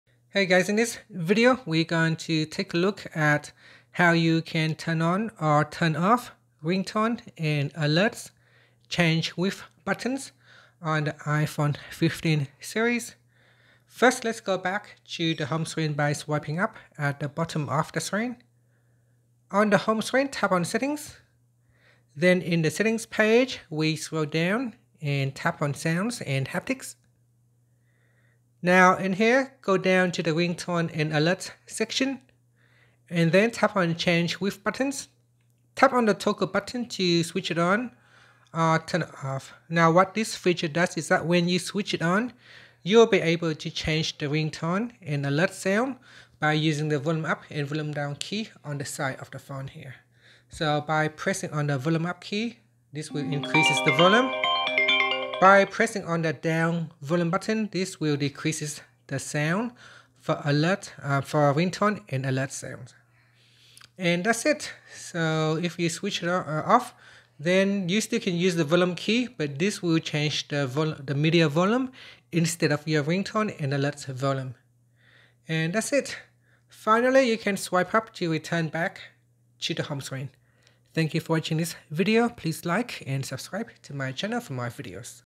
Maximize Convenience: How to Turn On/Off Ringtone & Alerts Change With Buttons on iPhone 15 Series
In this comprehensive guide, discover how to master the "Ringtone & Alerts Change With Buttons" feature on your iPhone 15, iPhone 15 Pro, 15 Pro Max, or iPhone 15 Plus. Learn how to customize your device's behavior when it comes to adjusting ringtones and alerts with the volume buttons. This powerful feature offers you greater control and convenience, enhancing your overall iPhone experience.

🔔 Key Topics Covered:

- Enabling and Disabling the "Ringtone & Alerts Change With Buttons" feature.

📱 Whether you're a new iPhone 15 user or looking to unlock the full potential of your device, this tutorial is your go-to resource.